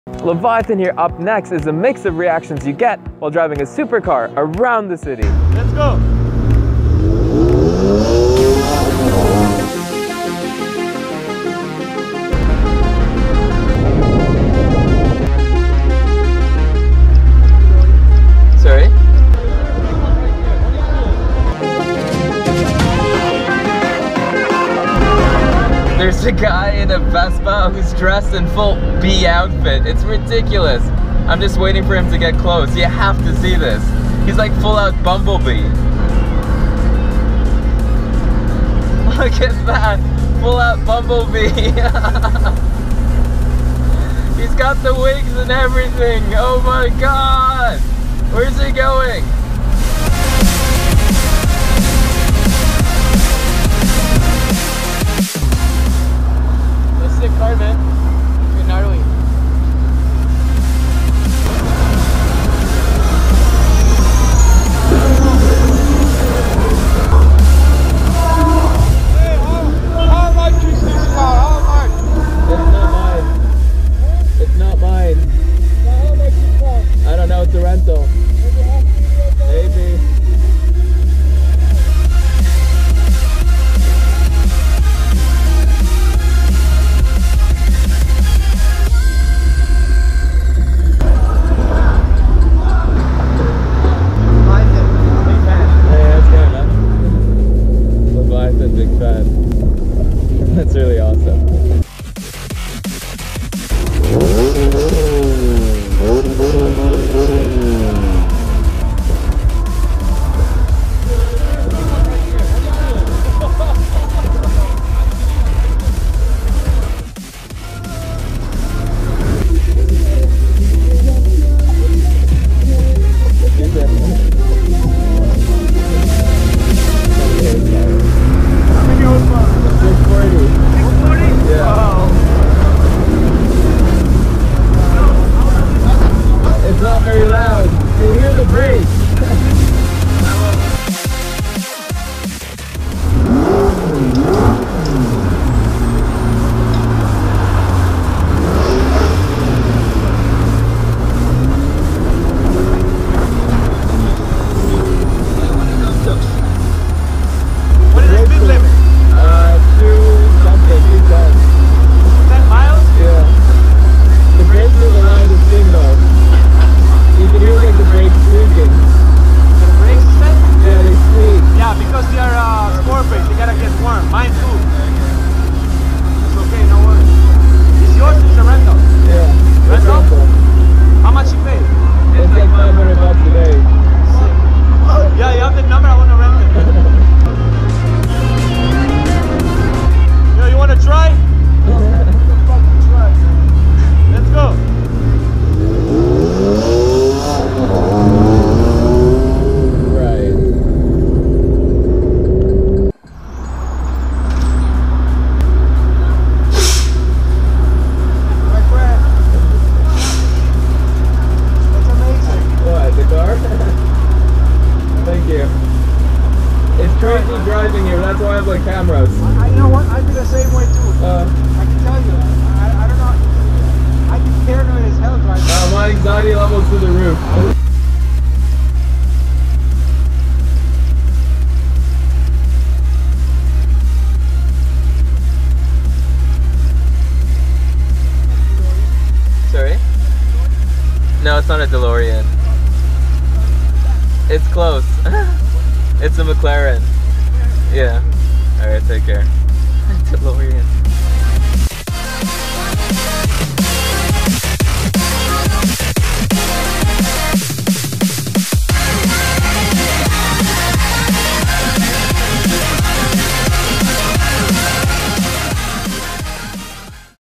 DRIVING A SUPERCAR: EPIC CITY REACTIONS! Wow a McDeloren P1!!!
Are you curious to know what it is like driving a McLaren supercar through the city? Here are a mix of supercar reactions!